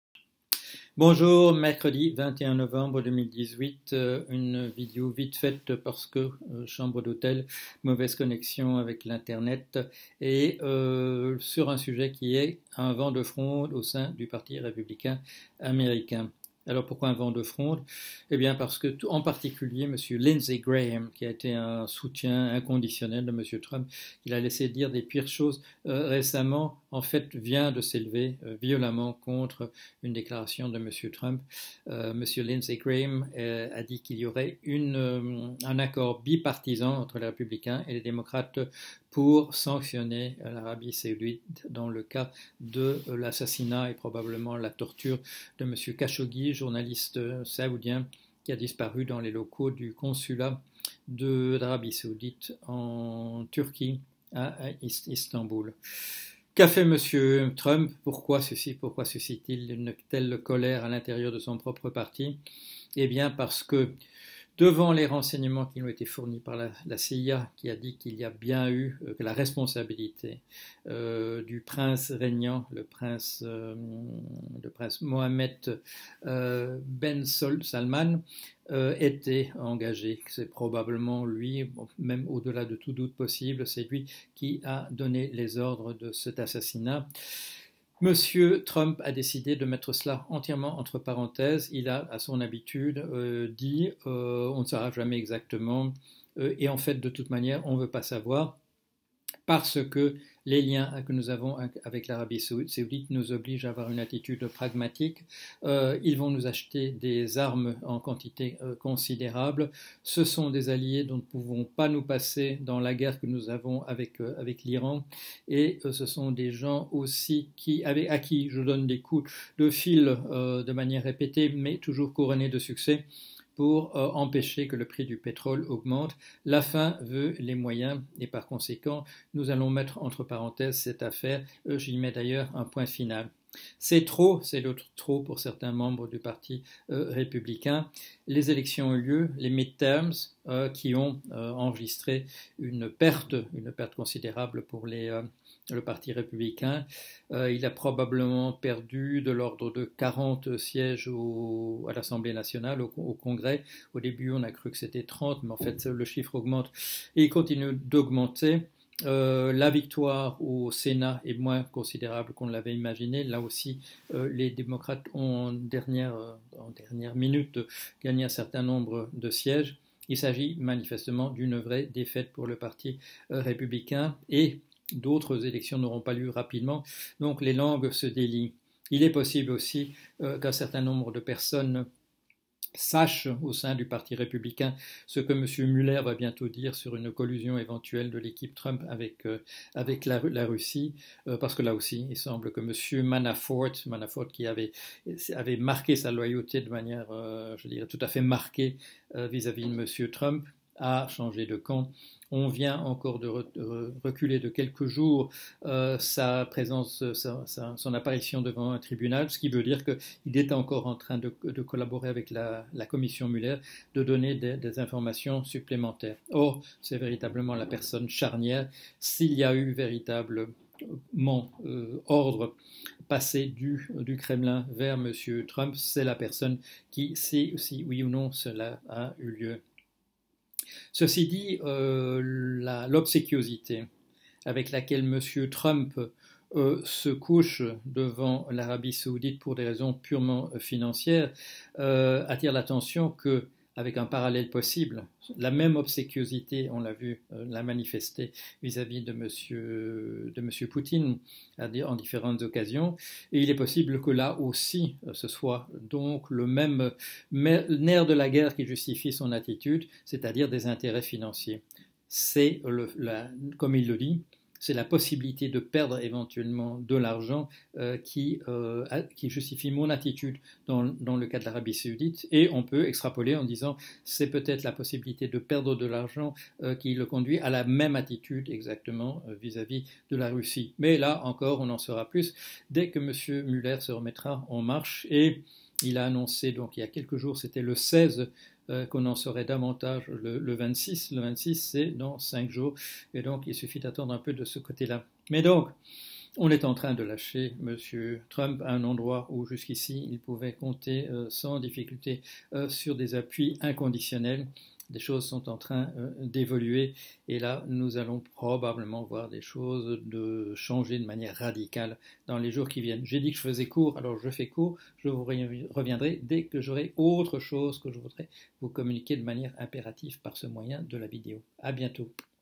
Paul Jorion - Un vent de fronde au sein du Parti républicain
Trump met un point final à l'affaire Khashoggi